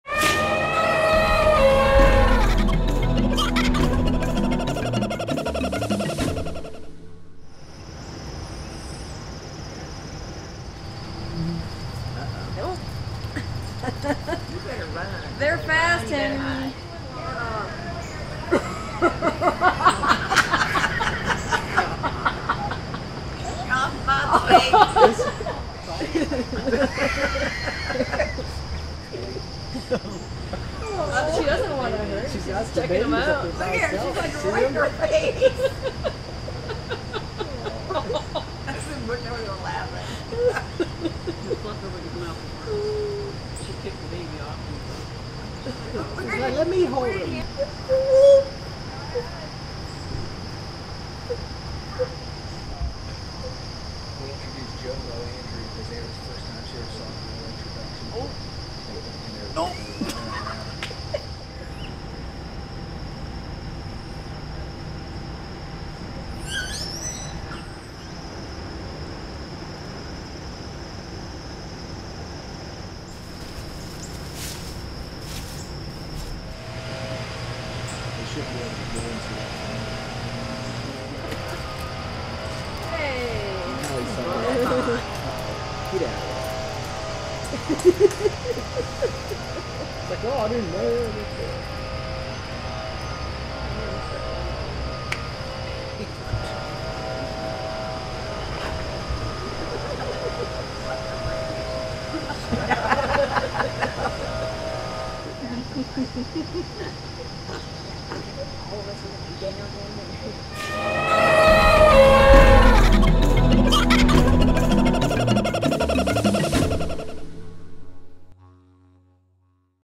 Orangutan Loves Gibbon Baby - Cincinnati Zoo
White-handed gibbons and Orangutans go on exhibit together for the first time in nearly 3 years. Lana, a female orangutan, gets up close and personal with the gibbon's 2 year old baby.